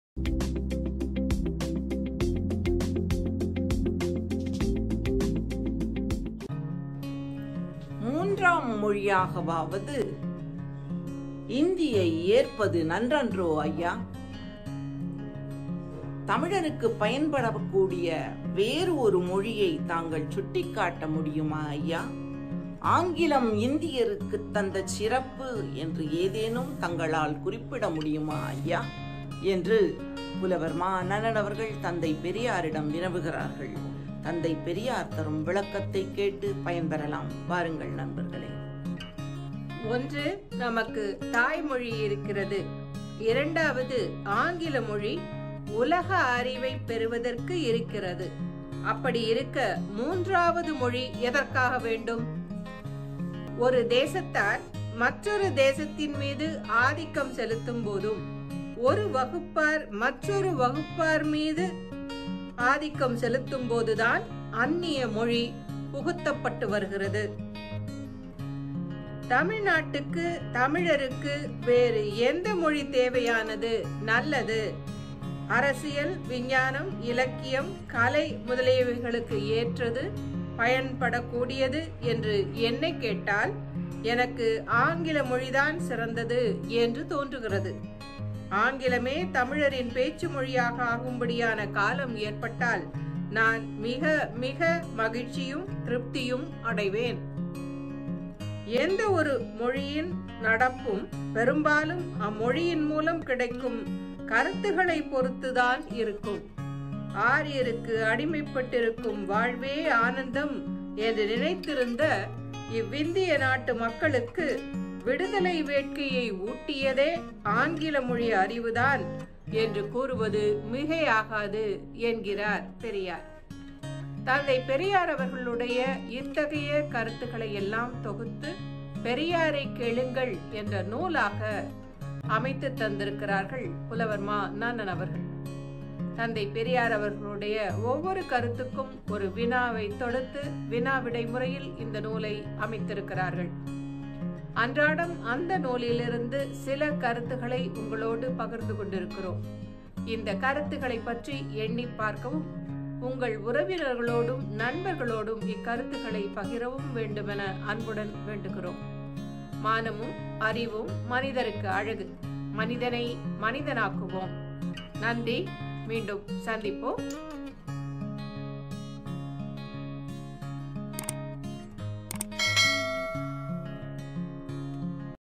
பெரியாரைக் கேளுங்கள் 562
பெரியாரைக் கேளுங்கள்/மா நன்னன்/மூன்றாம் மொழியாகவாவது இந்தியை ஏற்பது நன்றன்றோ? தந்தை பெரியார் விளக்குகிறார்